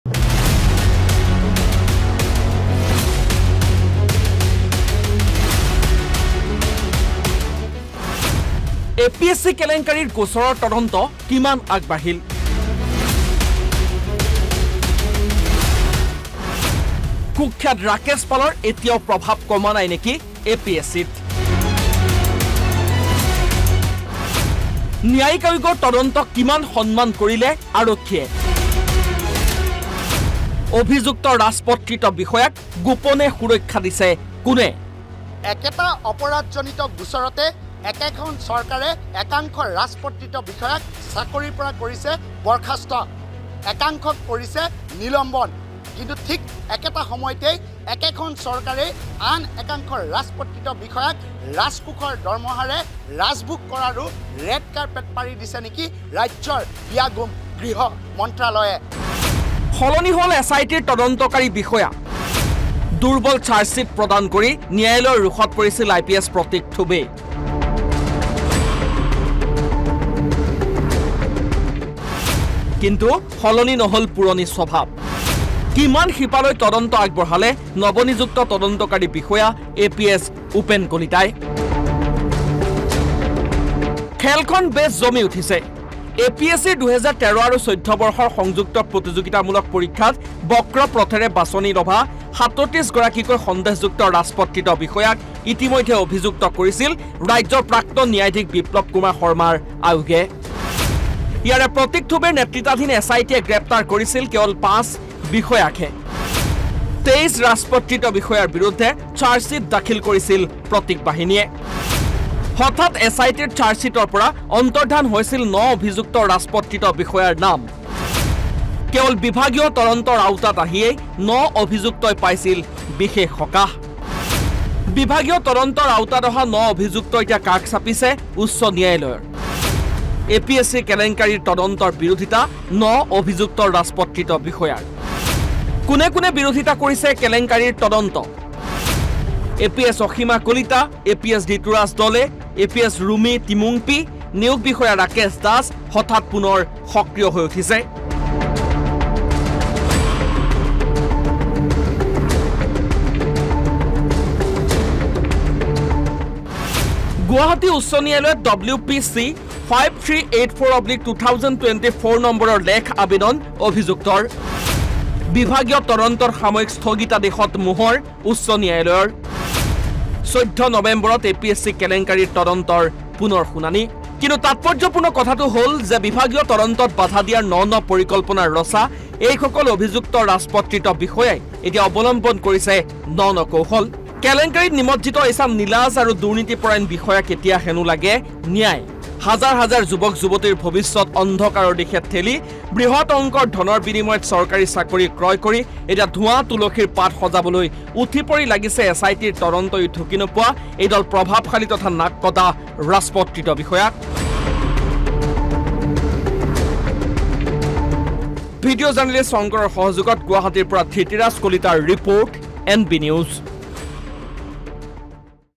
APSC কেলেংকাৰিৰ তদন্ত কিমান আগবাঢ়িল? কুখ্যাত ৰাকেশ পালৰ প্ৰভাৱ কমা নাই নেকি?
APSC কেলেংকাৰিৰ তদন্ত কিমান আগবাঢ়িল? কুখ্যাত ৰাকেশ পালৰ প্ৰভাৱ কমা নাই নেকি? ন্যায়িক আয়োগৰ তদন্তক কিমান সন্মান জনালে আৰক্ষীয়ে? অভিযুক্ত ৰাজপত্ৰিত বিষয়াক গোপনে সুৰক্ষা দিছে কোনে?